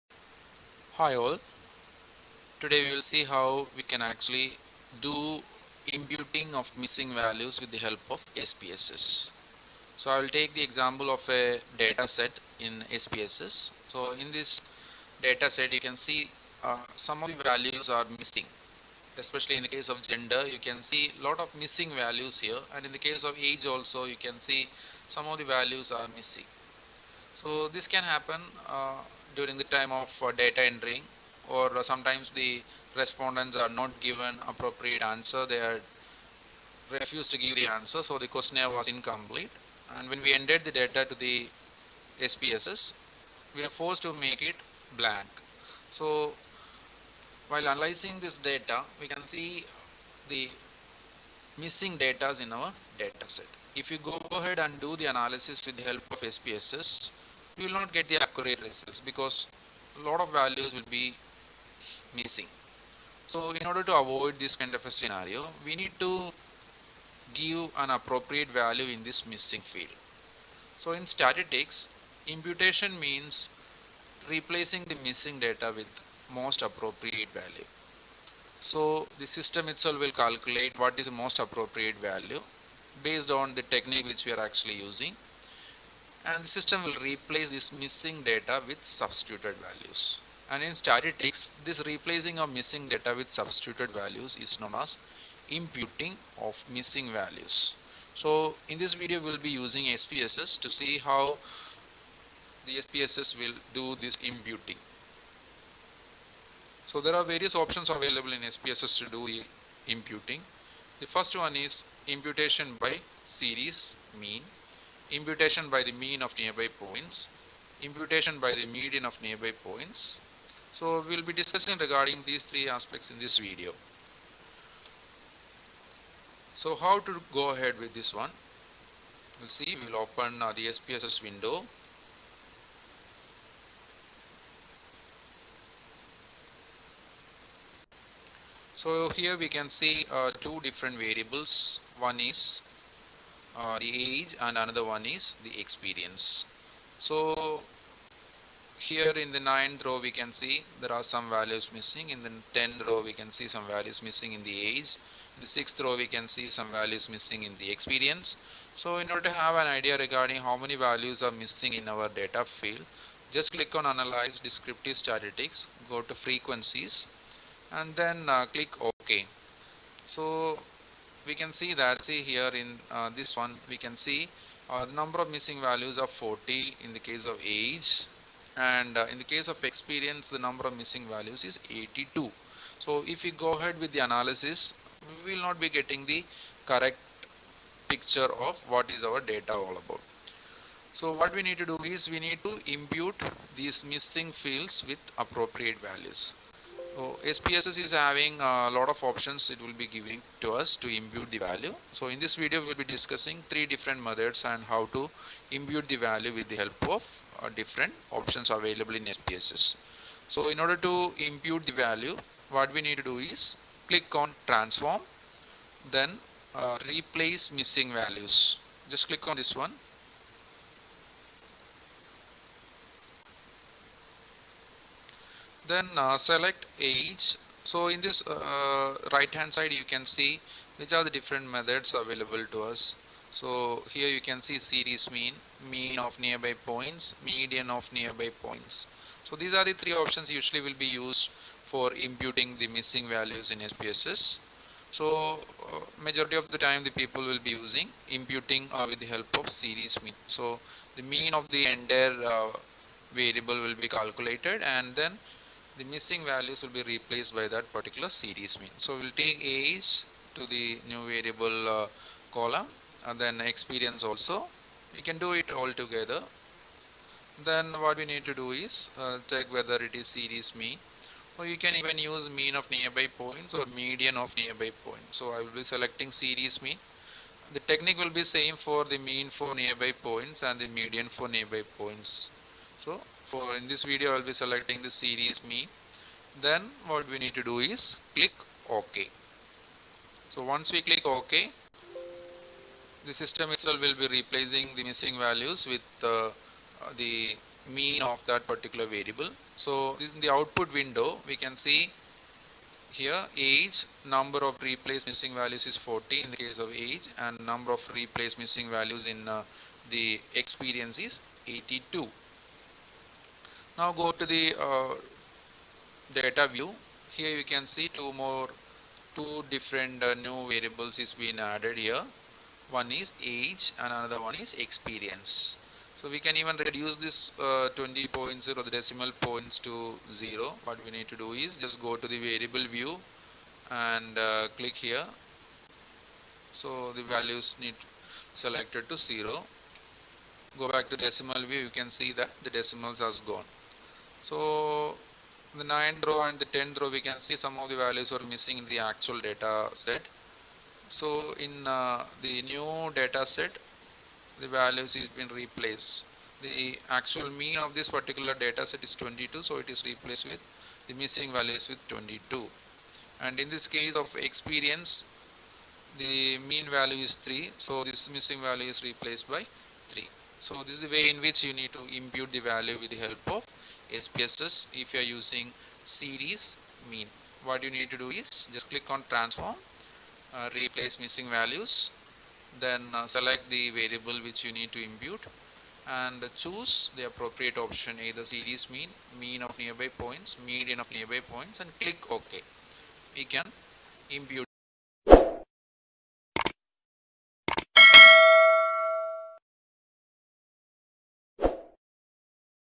Replacing missing values / Imputing Data In SPSS (Part-1)
Imputing missing data in SPSS
Imputation by Series mean
Imputation by Mean of nearby points
Imputation by Median of nearby points